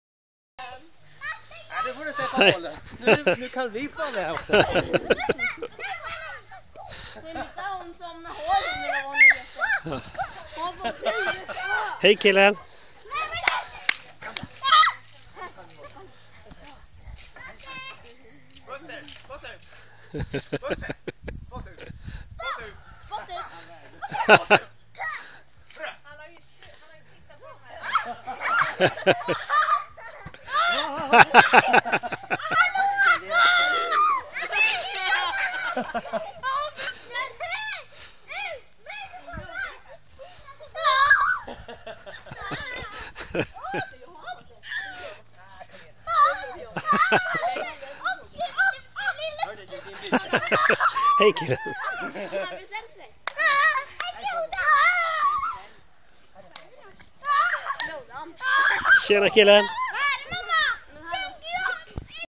Tillfälligt stopp i fotbollsmatchen när Buster snor bollen
Henriks återföreningsfest på Bäckaskog den 28 juli 2012. Matte och Johannas hund Buster hoppar in i matchen...